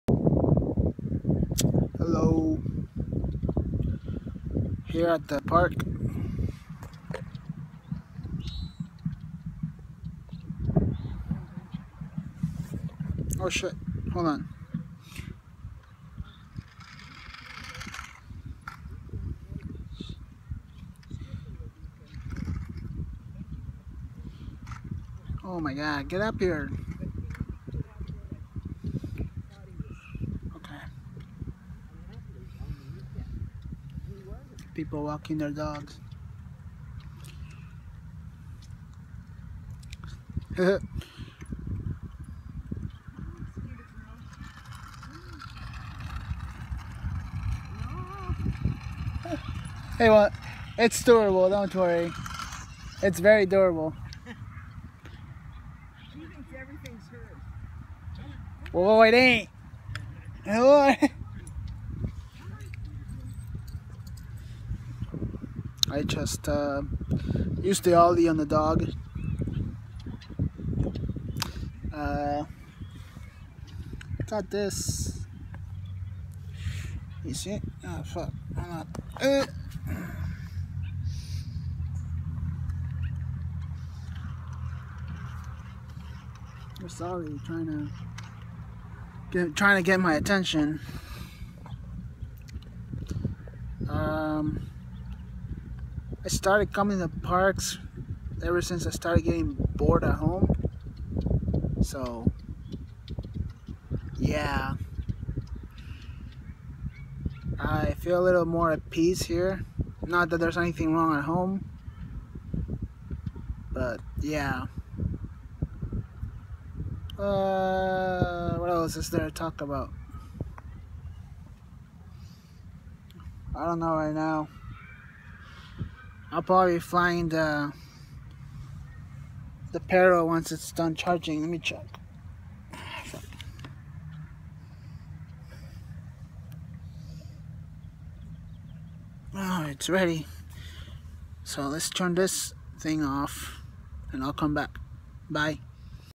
Park Solo Hangout
I started hanging out at regional parks by myself ever since last year. The reason for that is because I've started to feel the depression effects I get from hypothyroidism that I had ever since I was diagnosed with this curse. I don't enjoy it, I don't like it and the only way to escape is through doing these solo hangouts.

Where I am at now is the Cerritos Regional Park in Cerritos, California. Here I came to play with my Sphero Ollie and Parrot Swing Mini Drone. I'll probably be doing more of these as time allows.